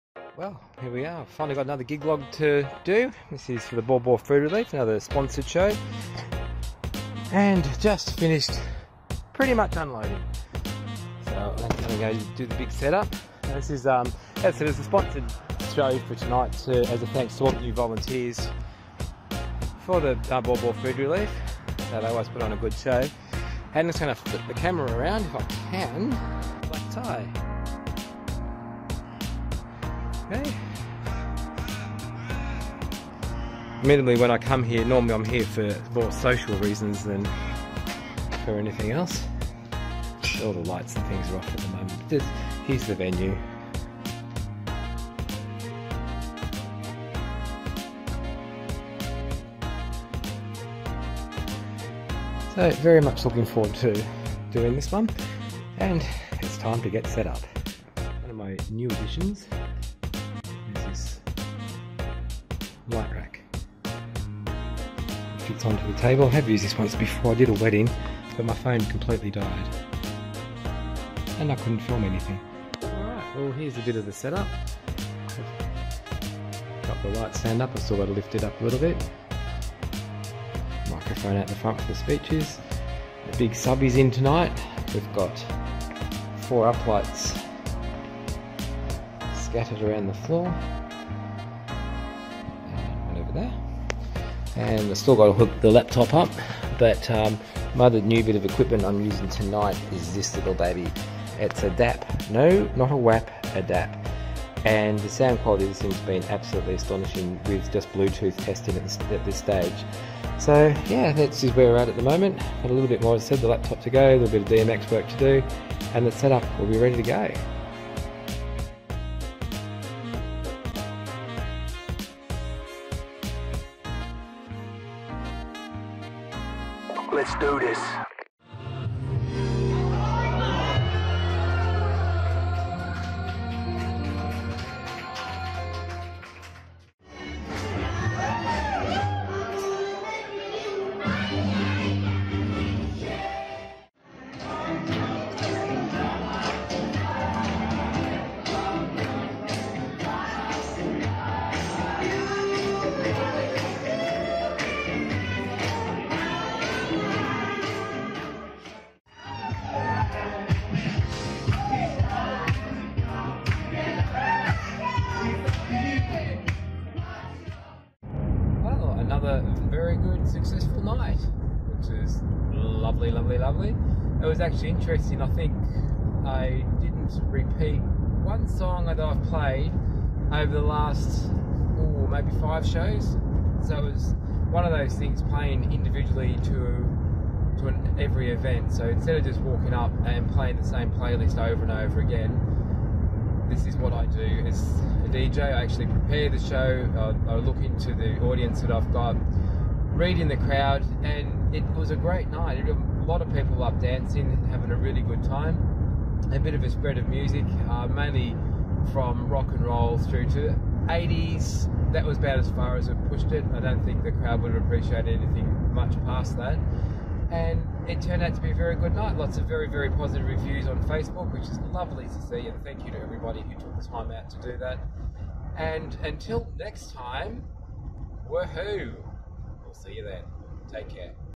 DJ Gig Log
Sponsored Show for the Baw Baw Food Relief as a big thank you for all their wonderful work and volunteers.